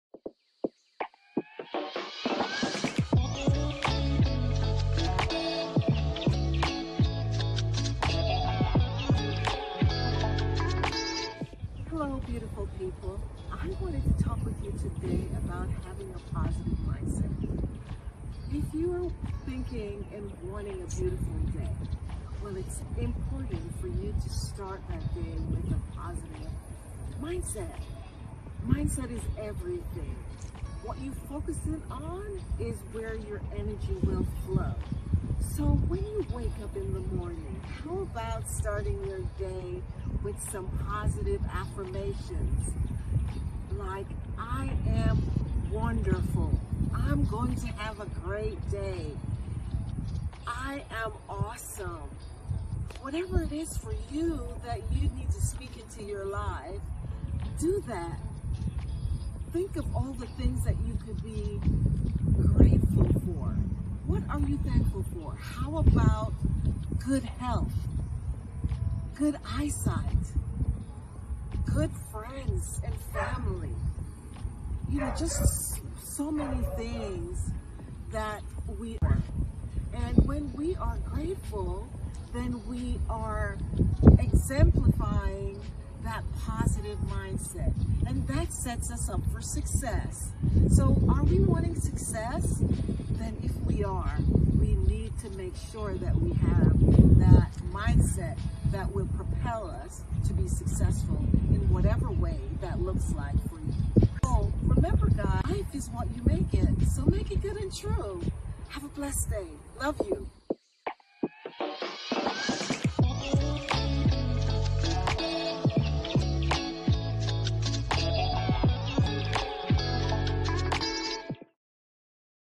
Angela Baptiste, MBA-DISC Consultant: "A Positive Mindset leads to positive days."
Inspire and Ignite your passion! motivating clients to be intentional with their leadership and personal growth plan becoming the best version of themselves by dispelling limiting beliefs and fear. Hire Angela to speak at your next event.